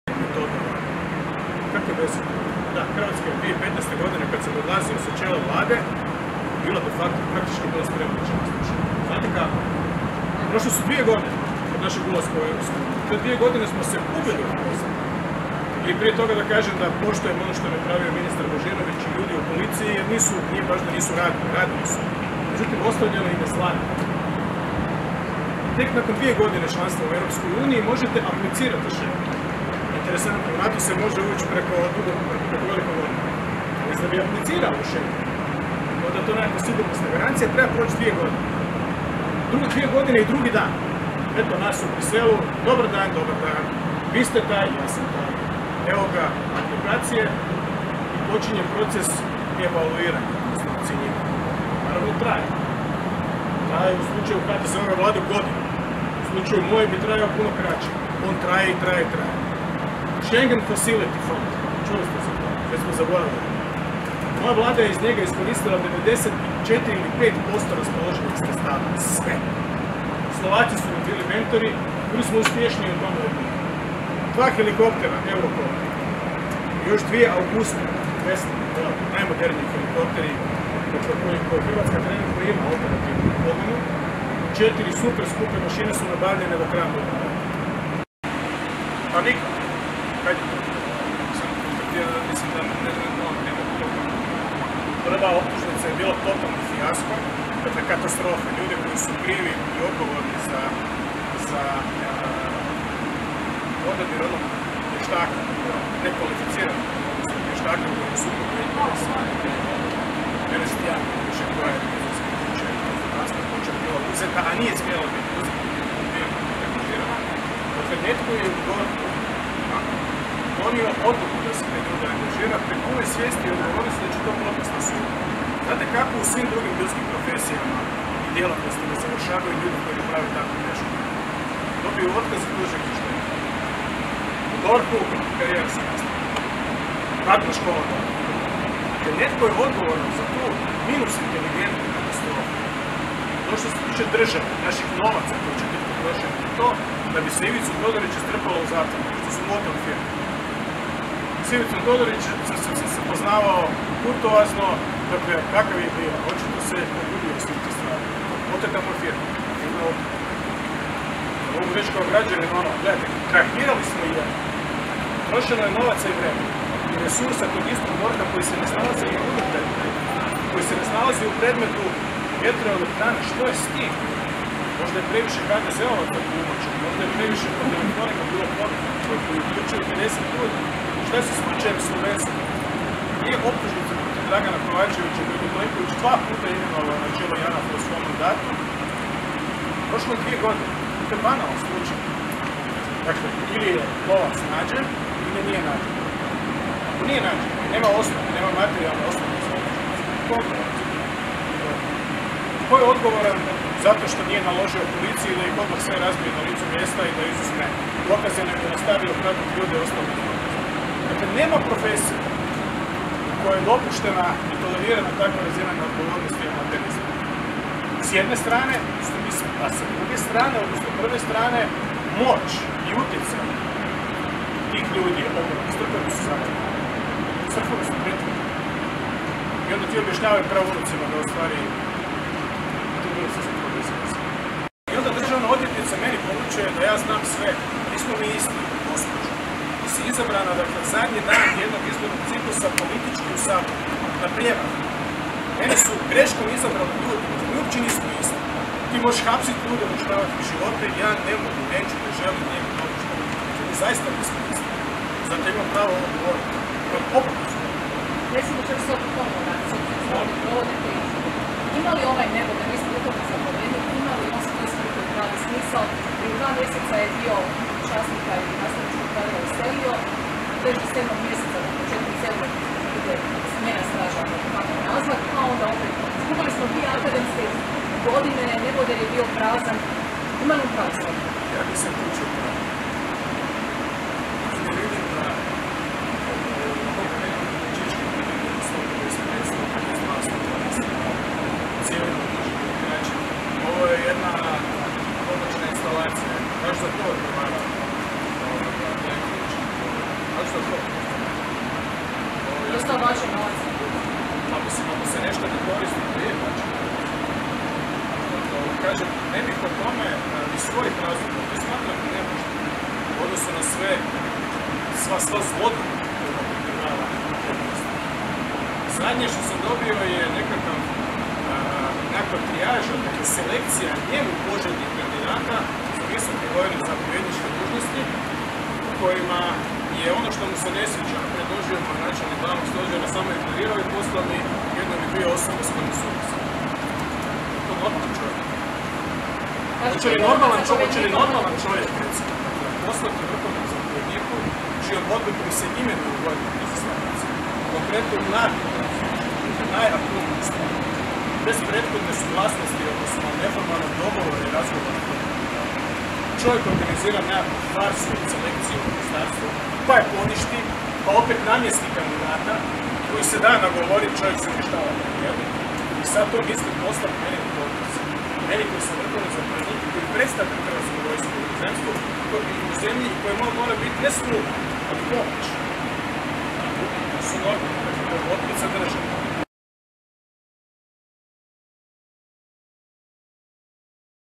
Milanović: Ivici Todoriću je oteta firma
Predsjednik i vrhovni zapovjednik Zoran Milanović sudjelovao na svečanoj promociji polaznika vojnih škola Hrvatskog vojnog učilišta "Dr. Franjo Tuđman“.